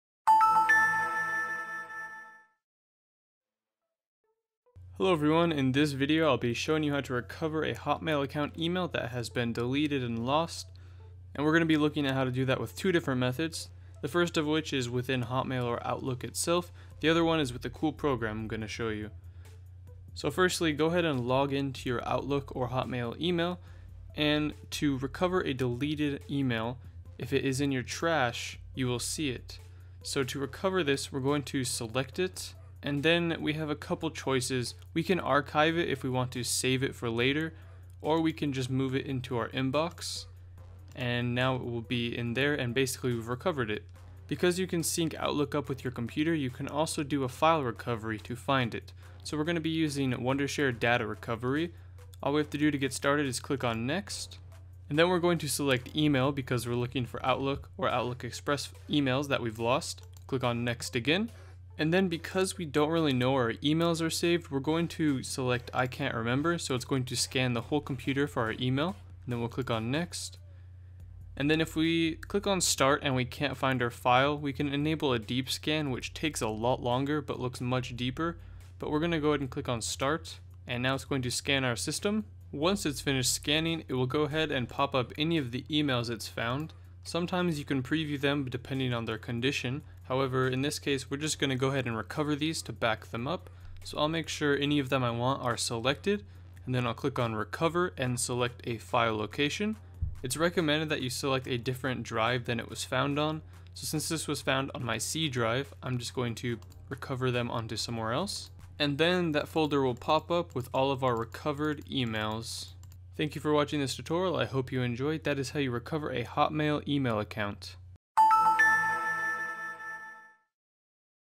How to Recover Deleted Emails from Hotmail Account
Hotmail is a quite popular email software over the internet, probably the most frequently used program. It's not rare a occasion if you delete some important emails accidentally.
·  Keep calm. Just don’t freak out. You should know that every deleted files are hidden somewhere inside your computer until they are overwritten, so being nervous won’t help you.
·  Check the Trash folder to make sure if it is still there. There are cases that the deleted files are stored in the Trash folder.
·  Find a software on the Internet that is designed to help you get back your deleted emails. In this video we demonstrate how to follow these steps to get the lost emails back.